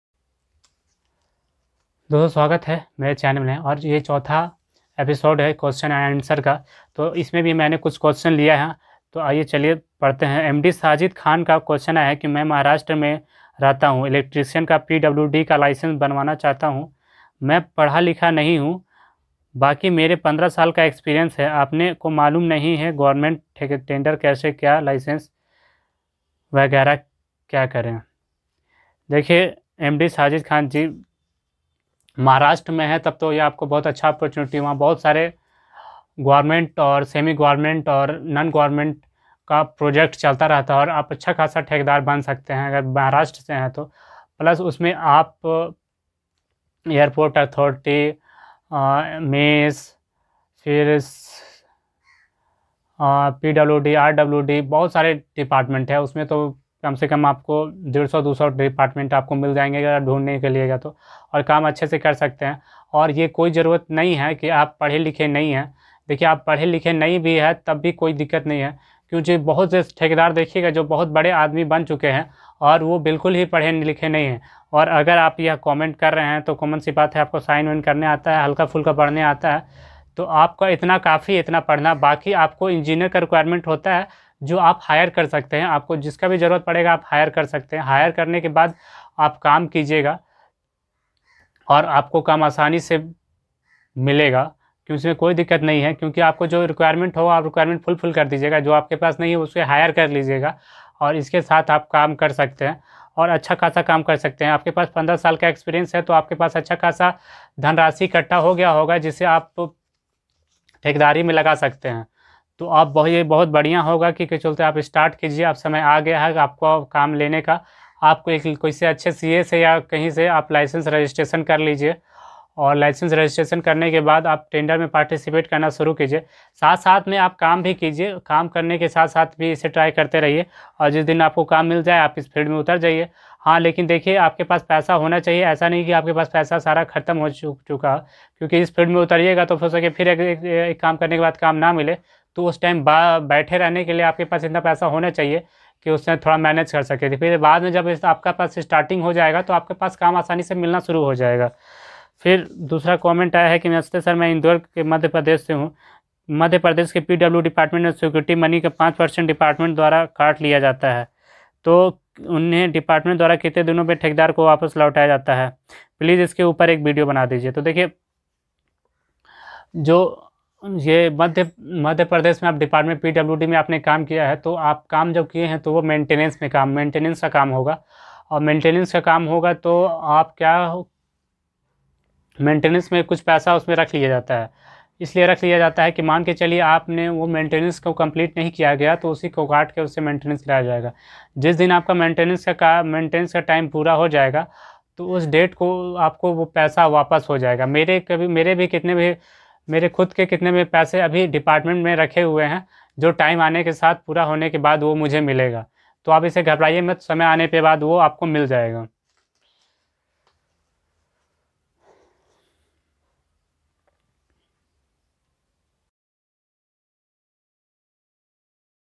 Pwd ka licensekaise banye ||4th ठेकेदारी पर आधारित क्वेश्चन एंड आंसर| thekedari kaise, thekedari
dosto swagat hai mere channel civil bazzar2.0 me दोस्तों आज के वीडियो में मैंने आपके के द्वारा भेजा गया कुछ क्वेश्चन और उसके जवाब लेकर आया हूं तो वीडियो को अंत तक देखें और हमारे टेलीग्राम चैनल को व्हाट्सएप ग्रुप को और मेरे इंस्टाग्राम प्रोफाइल को जरूर लाइक फॉलो करें